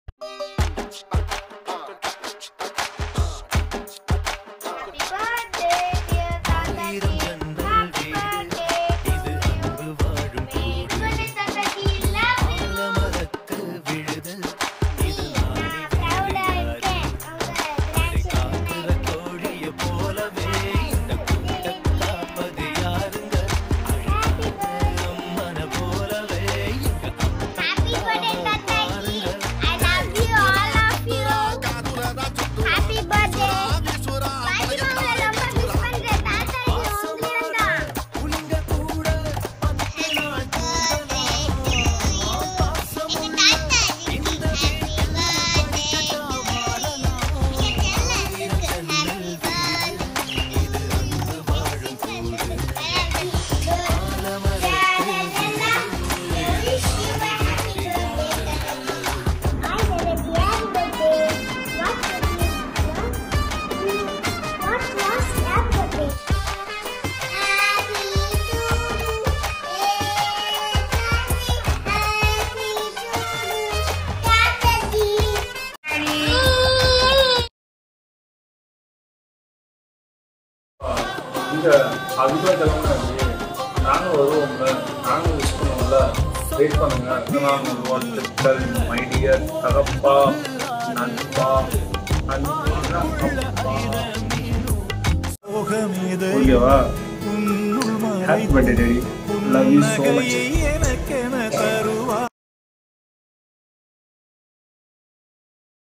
Birthday wishes video to my Dady10/08/2020
Hi buddies,

😍Happy Birthday To My Cool Dad. It's your special day! Let's celebrate! Another year older, but you're still a Handsome Rock Star🌟❤️